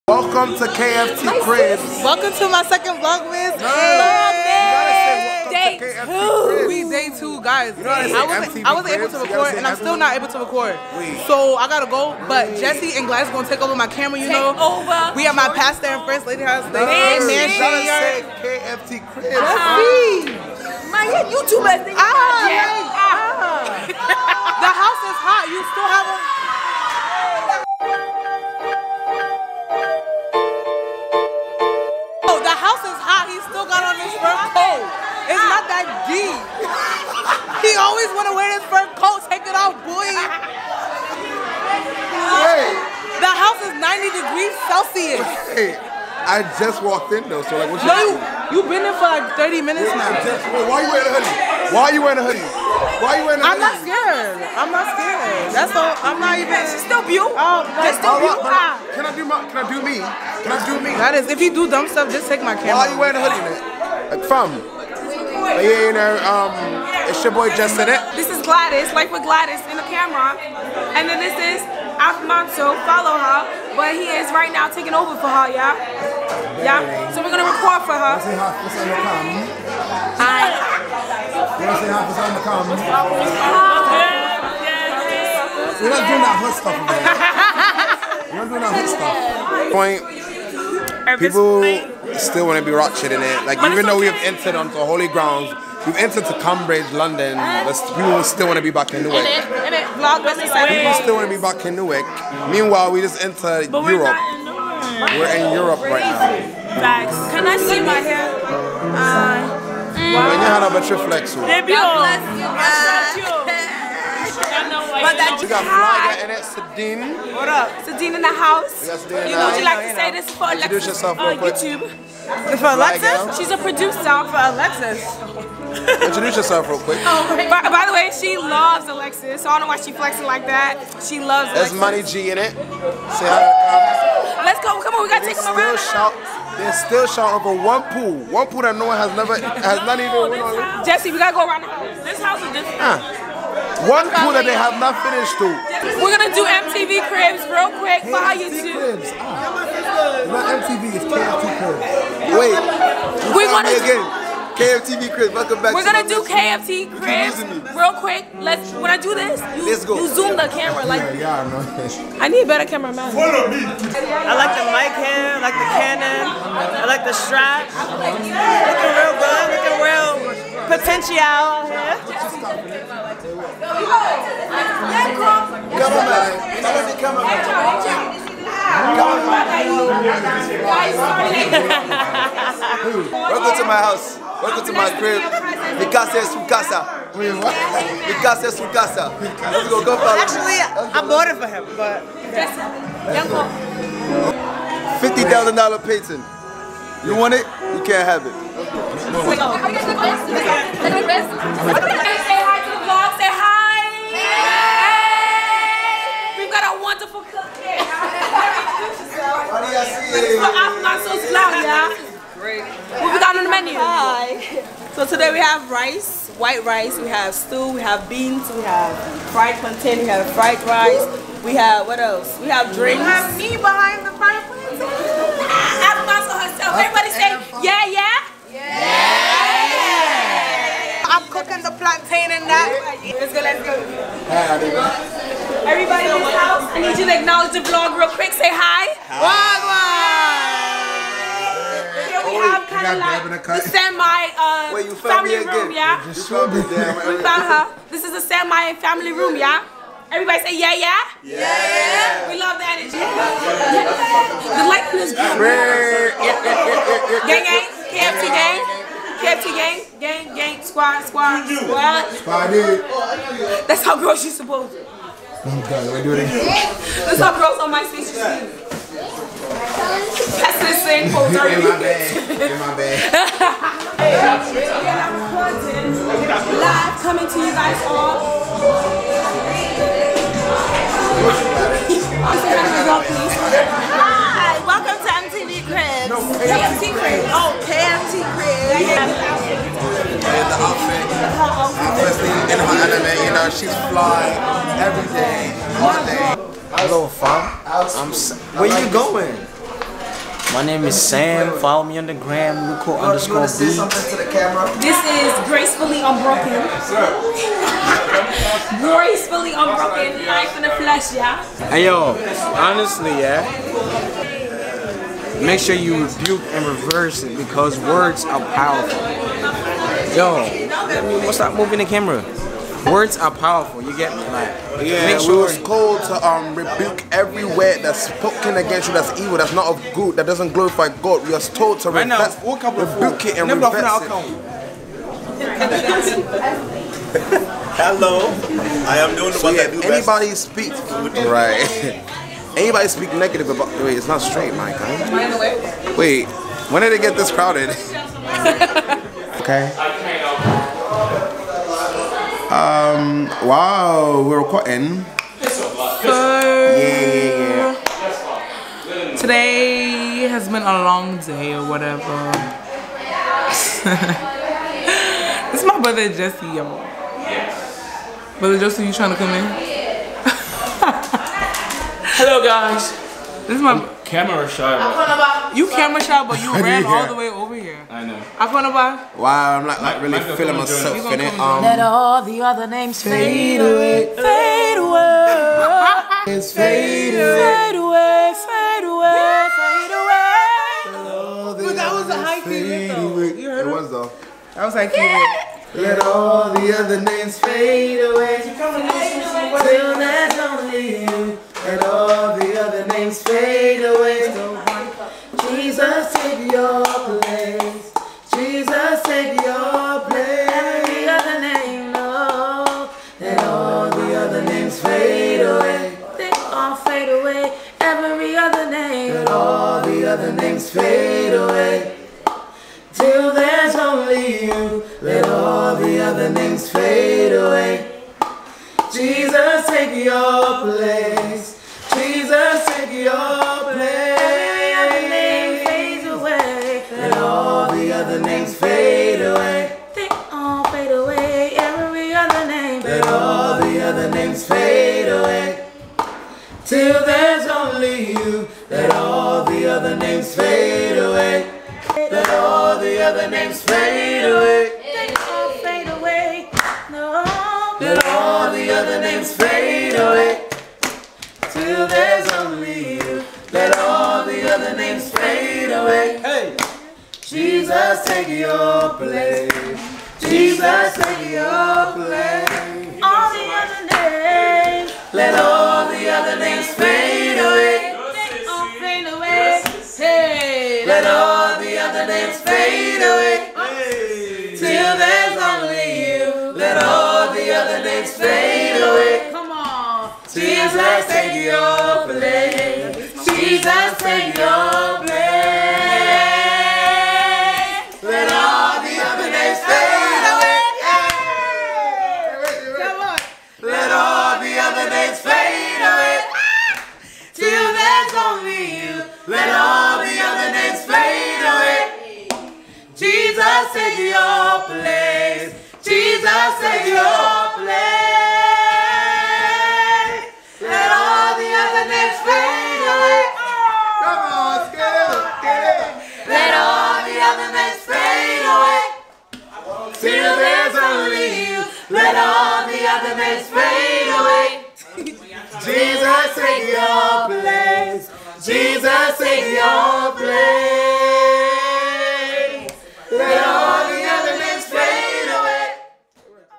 Sunday Funday | VLOGMAS DAY 2
Gladys, Jesse and a bunch others in this video will attempt to speak in a UK accent. Please forgive them!
PS as you can tell JESSE IS MAD ANNOYING

Snapchat afmanso